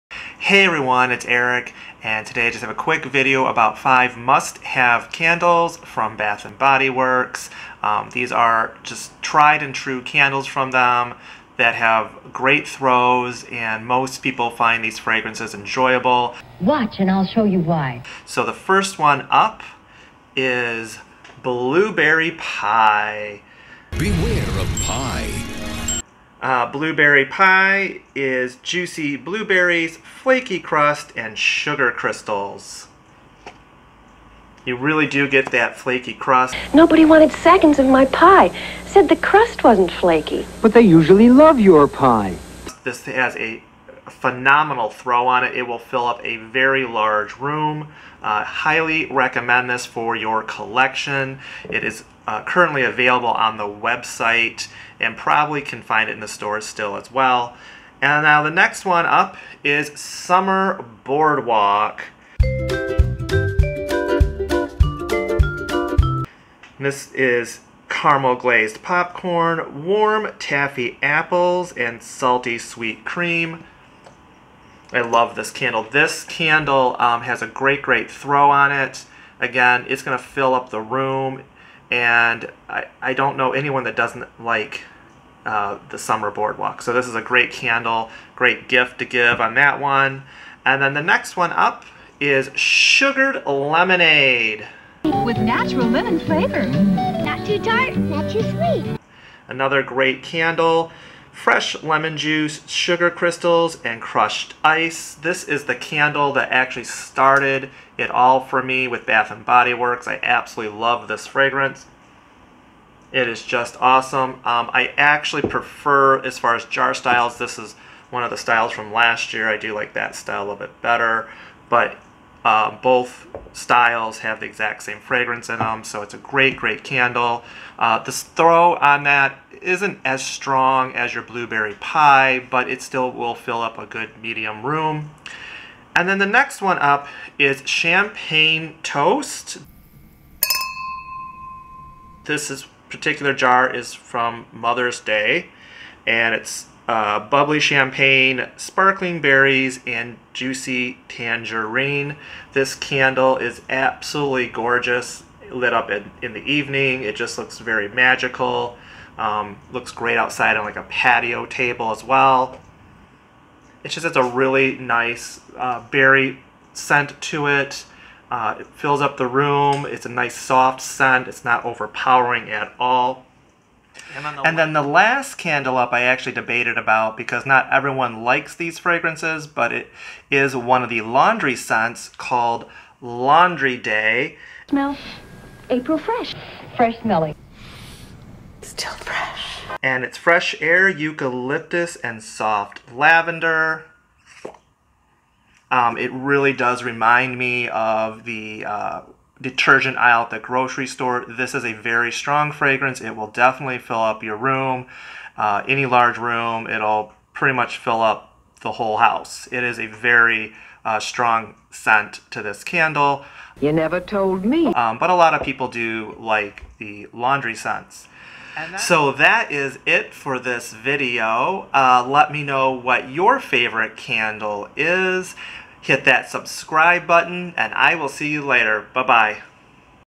5 Must-Have Bath and Body Works Candles
Five Bath and Body Works candles with a great throw for any room in the home. These five also make great house warming gifts!